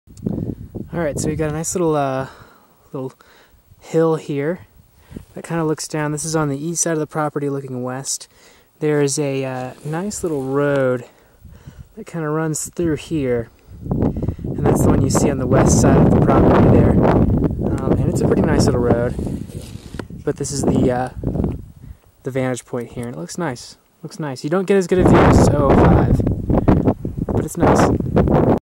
Peach Springs Arizona - 40 Acres for sale APN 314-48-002. Lowest price in area for beautiful and accessible land.